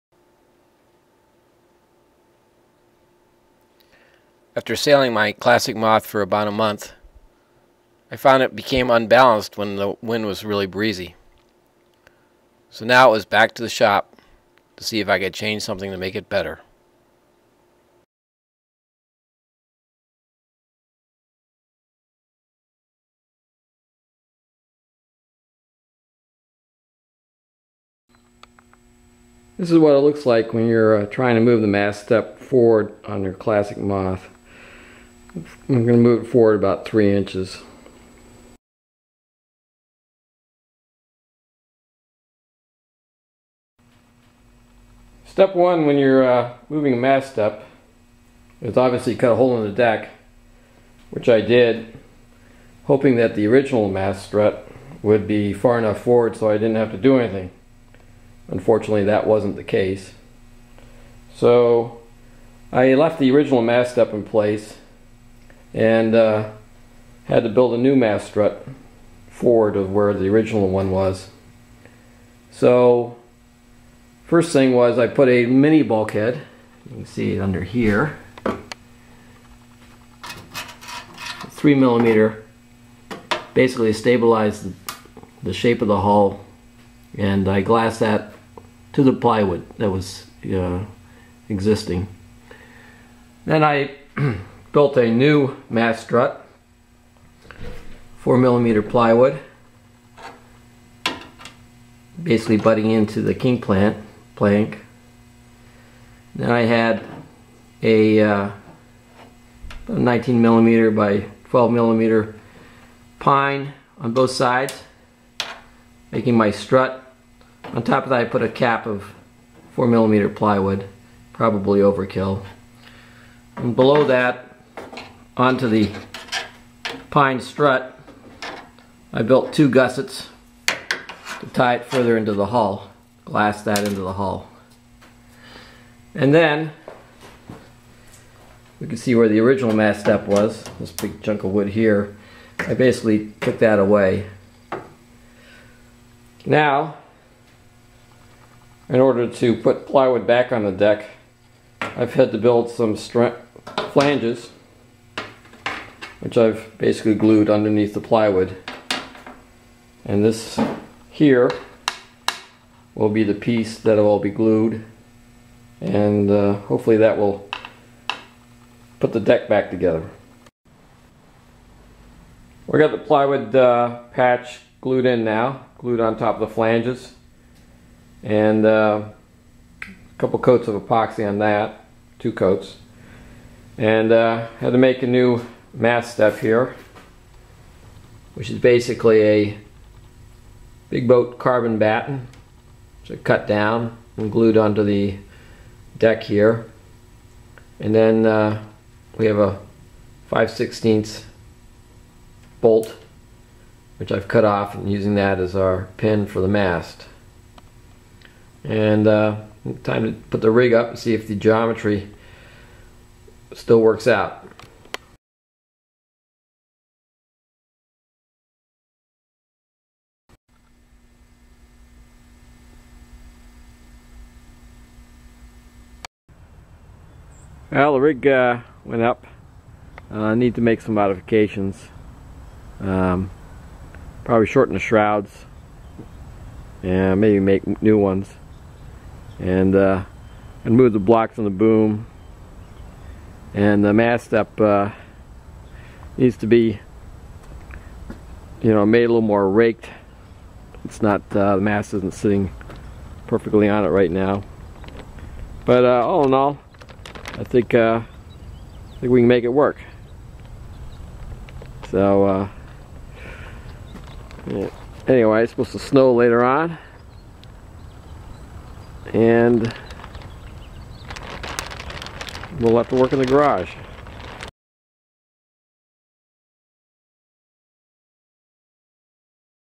Boatbuilding: Classic Moth - Maststep Modification
Moving the mast step forward on a Classic Moth.